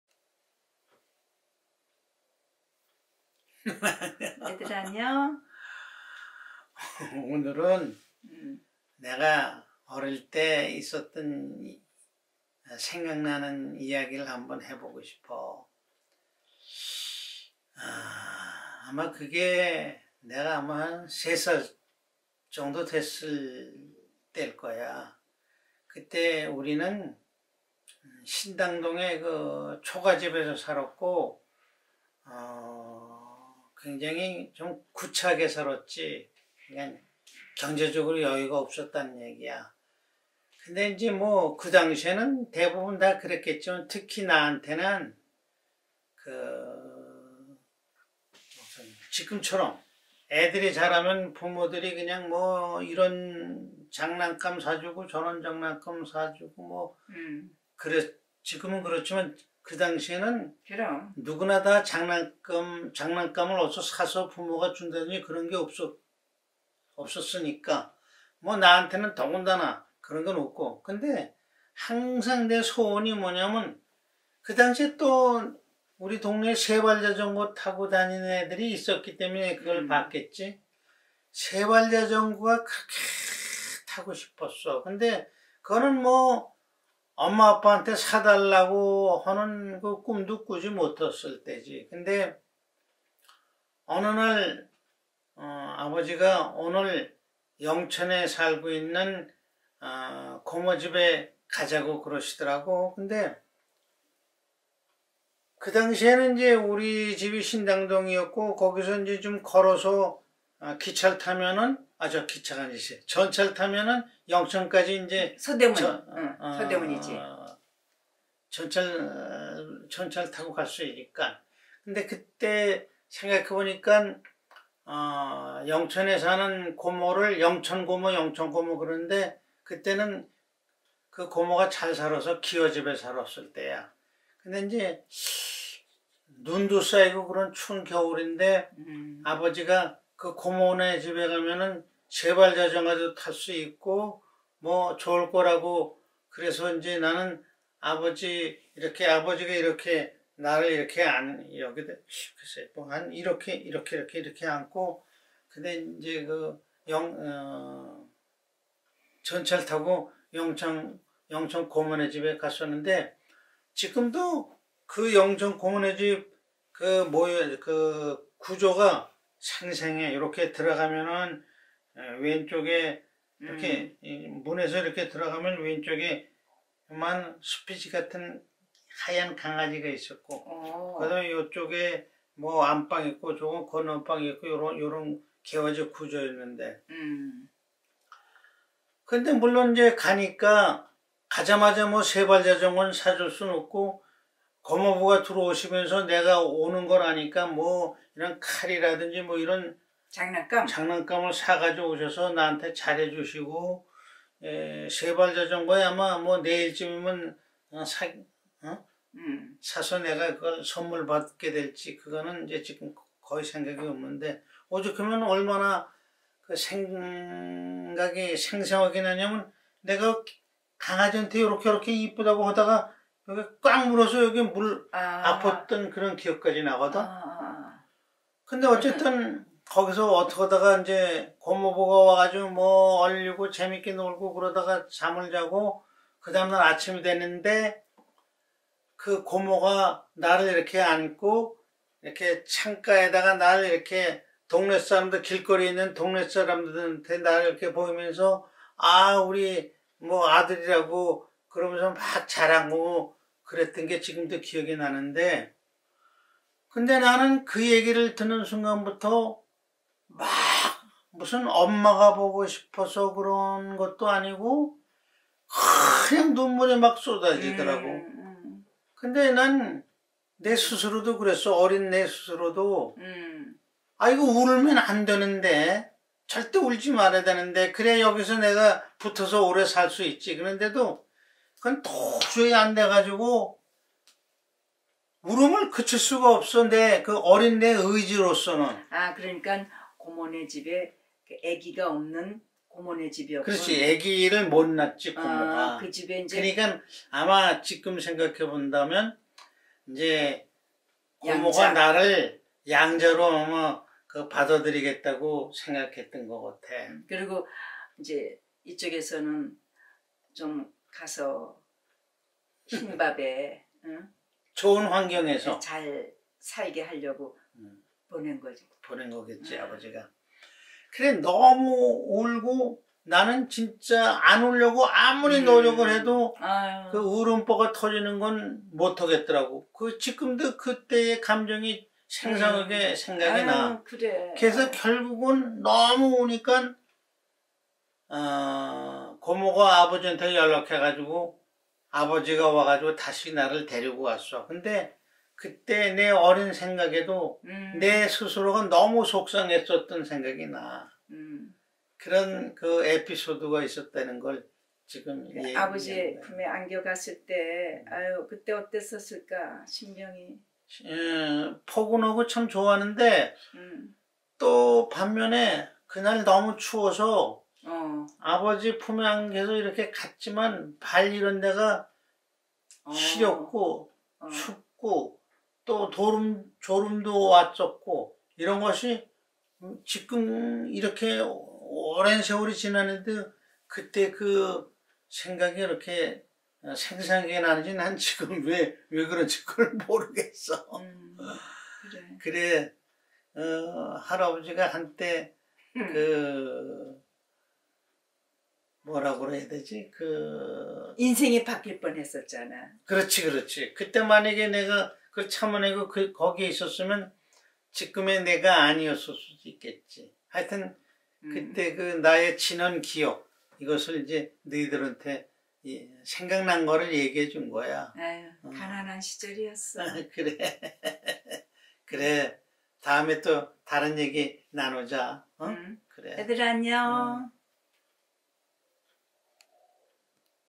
Dear Grandchildren - Tricycle (a sad memory)
The son of a poor family was taken as a son to a childless aunt. His mother gave him the excuse of a tricycle that her son wanted so much, but in fact the real reason was that at his aunt's house, the boy could make three meals a day full of white rice. The father took his son to the house of his sister, who lived well. He turned around thinking it is probably the best he can do for his son. The son as a child burst into tears. As much as he held on, he couldn't stop crying. No matter how patiently he tried, he didn't stop. The son still remembers everything sadly.

가난한 집 어린 아들은 애 없는 고모네 양자로 가게 되었습니다. 엄마는 아들이 갖고 싶어한 세 발 자전거를 앞세웠지만 사실은 세 끼 흰밥 배불리 먹을 수 있다는 게 이유의 전부였을 겝니다. 아버지는 아들을 데리고 잘 사는 누이네에 갑니다. 누이네한테 아들을 준다는게 어쩌면 다행이라 생각하고 돌아섭니다. 어린 아들은 눈물이 쏟아졌다고 합니다. 흑흑!!! 아무리 참으려 해도 멈춰지지 않았답니다. 아들은 그게 너무도 안타까웠다고 기억하네요.